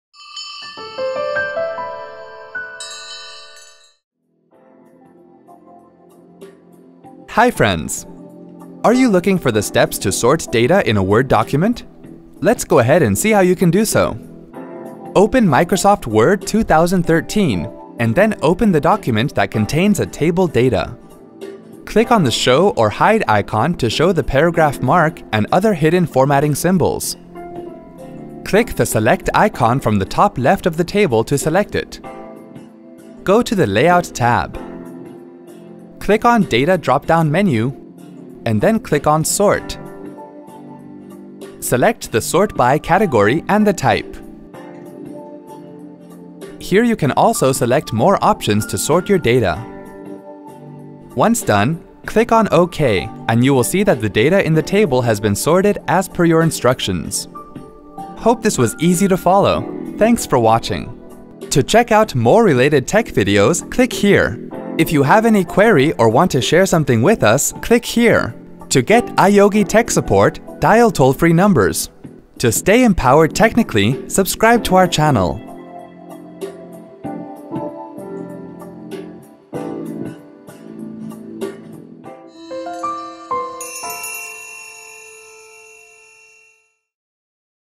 How to sort the rows of a tables in a MS Word :Tutorial
Are you looking for the steps to sort data in a Microsoft® Word document? Follow the steps given in this video to sort table data in a Microsoft® Word document.